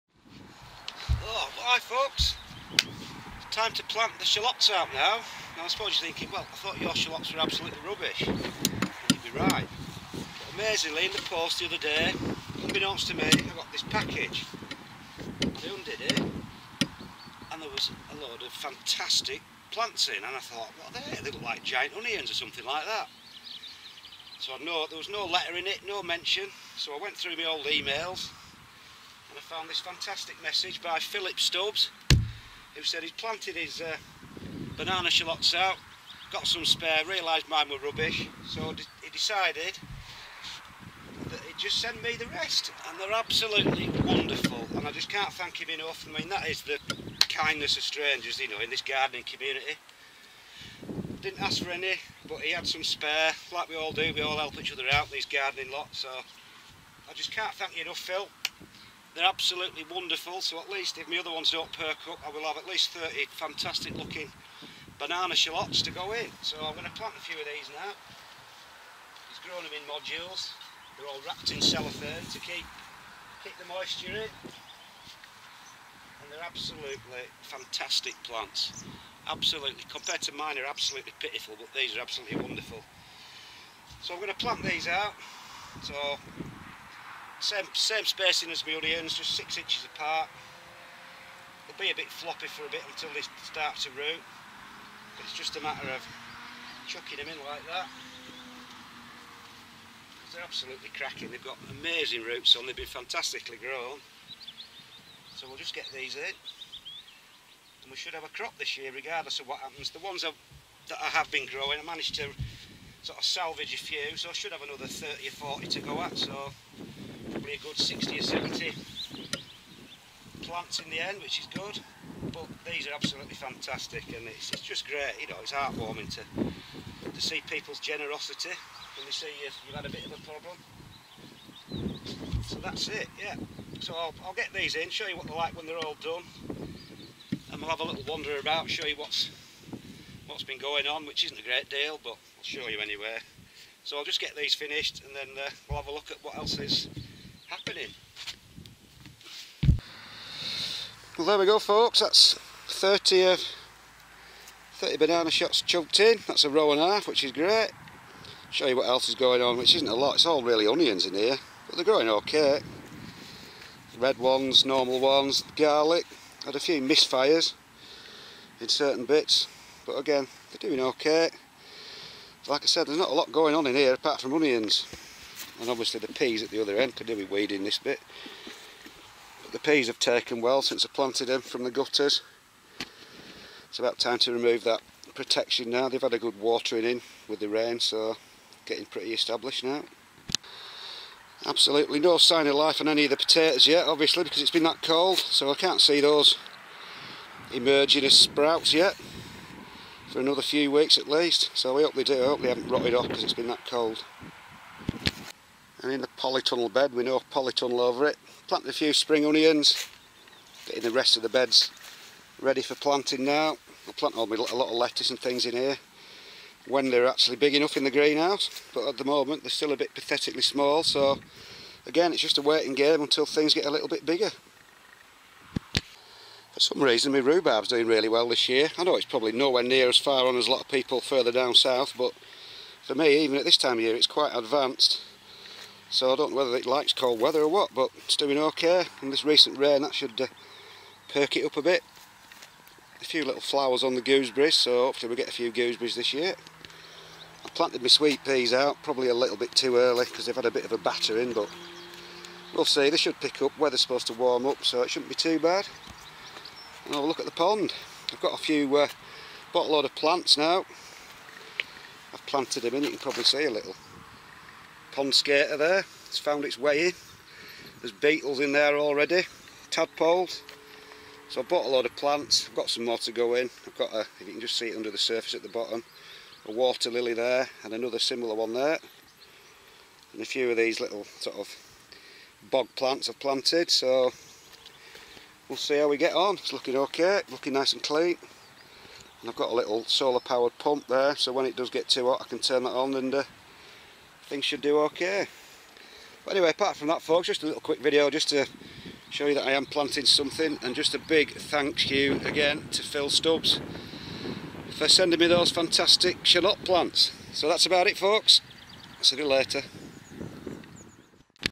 Allotment Diary : Planting Banana Shallots & Quick Tour
Planting out the Zebrunne Banana Shallot plants that were kindly sent to me.
A quick look around the plot to see what's going on.
The onions and garlic are growing slowly but steadily as are the peas.
No sign of life with the container grown potatoes yet .